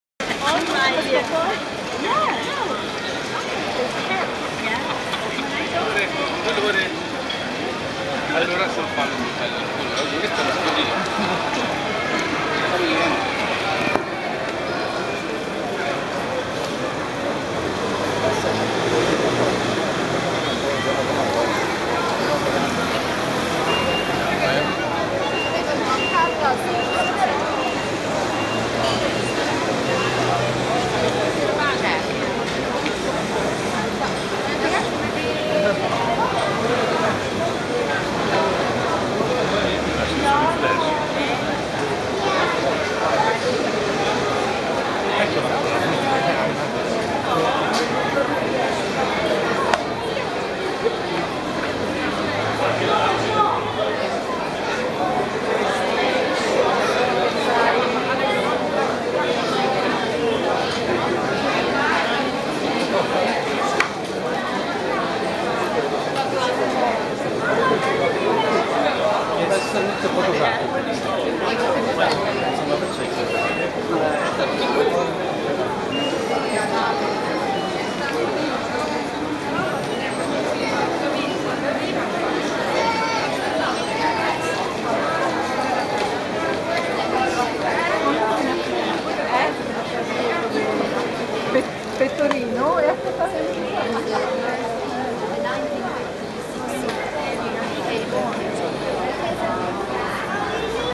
Florence - Ponte Vecchio - Walkthrough 1
Gang über die mit Läden und Häusern bebaute Brücke Ponte Vecchio über den Fluss Arno (Teil 1).

Walk over the built-up bridge "Ponte Vecchio" which lots of stores, mostly jewellery. (part 1)